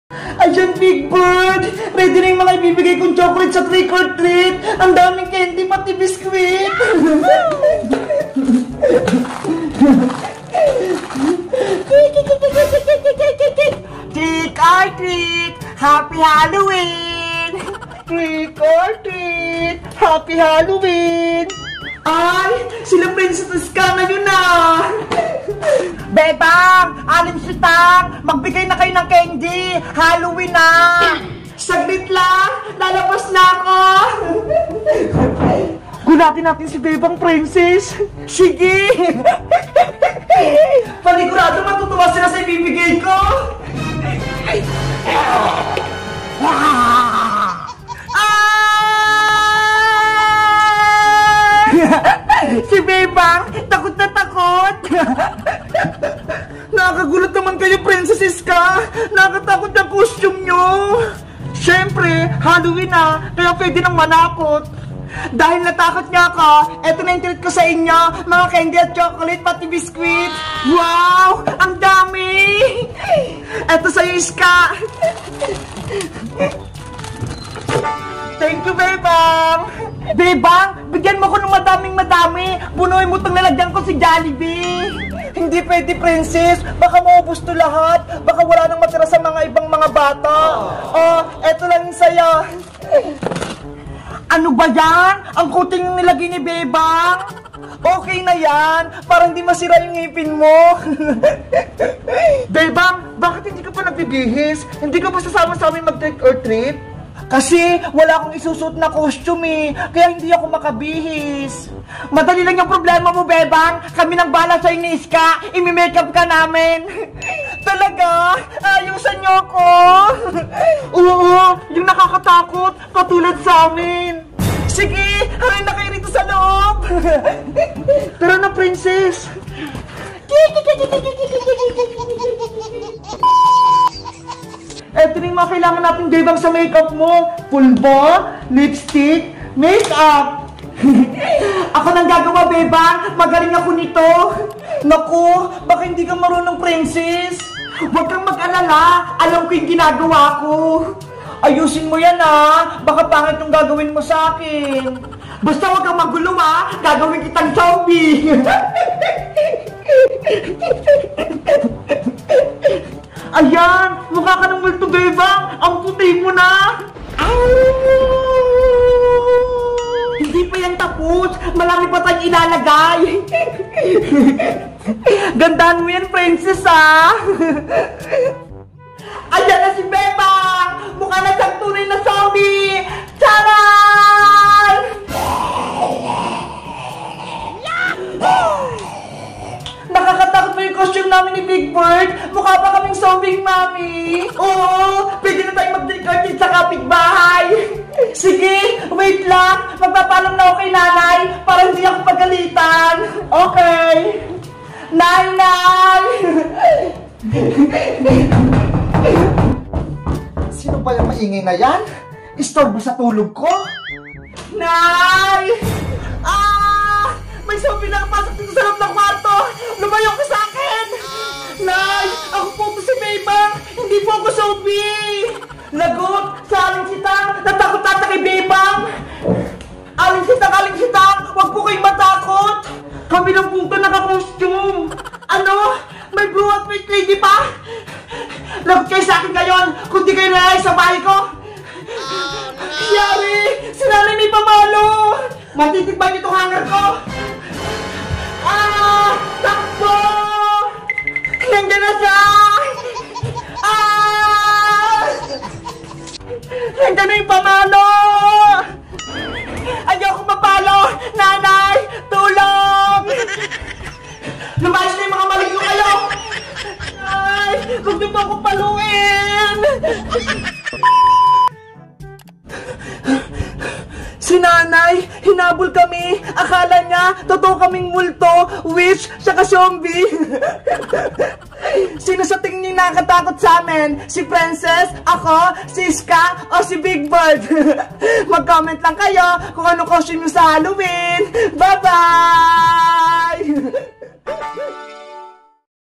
Halloween Costume sa Trick or Treat | Madam Sonya Funny Video
Walang Halloween Costume si Bebang. Mabuti tinulungan siya nina Prencess at Iska maging zombir